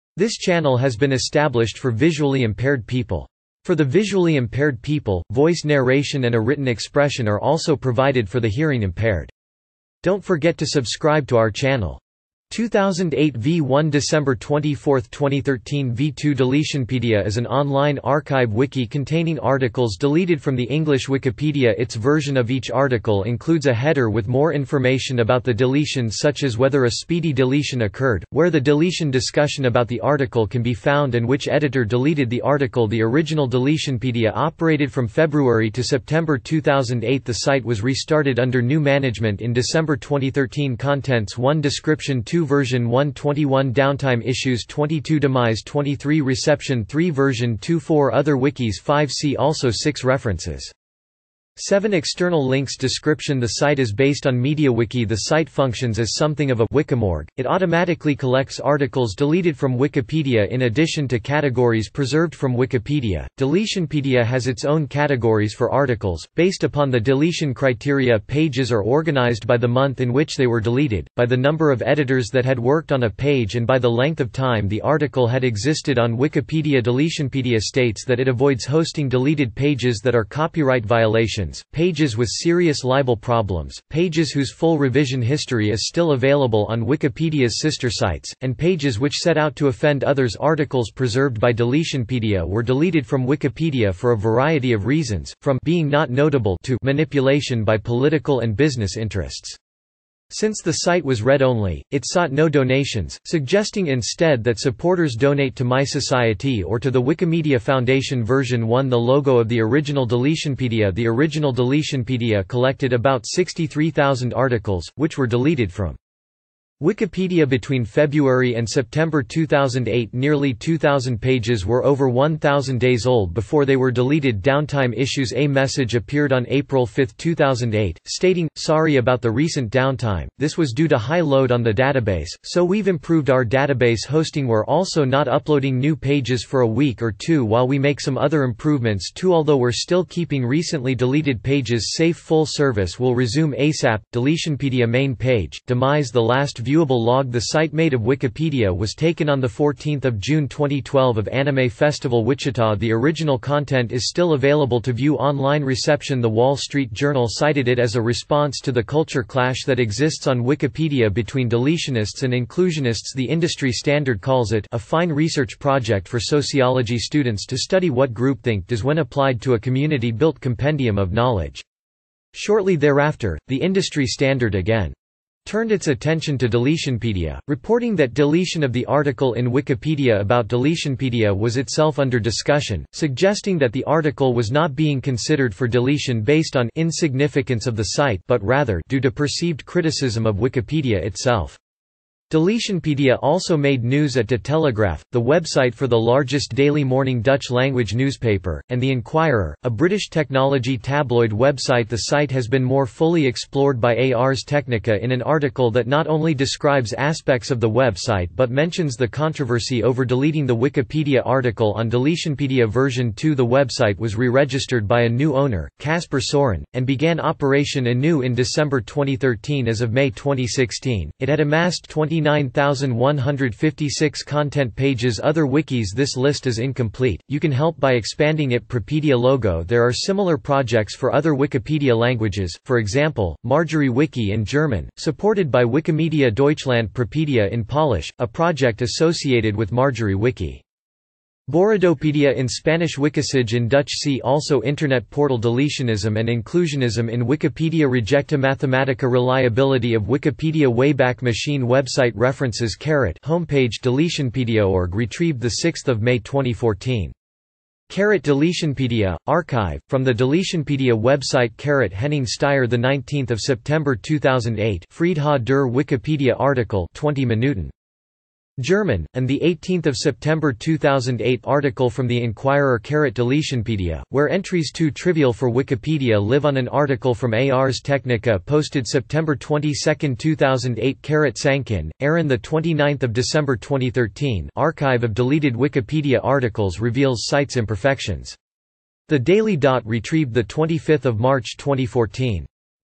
Deletionpedia
2008 v1
December 24, 2013 v2
Deletionpedia is an online archive wiki containing articles deleted from the English Wikipedia Its version of each article includes a header with more information about the deletion such as whether a speedy deletion occurred, where the deletion discussion about the article can be found and which editor deleted the article The original Deletionpedia operated from February to September 2008 The site was restarted under new management in December 20131
Contents
1 Description
2 Version 1
21 Downtime issues
22 Demise
23 Reception
3 Version 2
4 Other wikis
5 See also
6 References
7 External links
Description
The site is based on MediaWiki23 The site functions as something of a "wikimorgue"; it automatically collects articles deleted from Wikipedia4
In addition to categories preserved from Wikipedia, Deletionpedia has its own categories for articles, based upon the deletion criteria Pages are organized by the month in which they were deleted, by the number of editors that had worked on a page and by the length of time the article had existeddeletionpedia Deletionpedia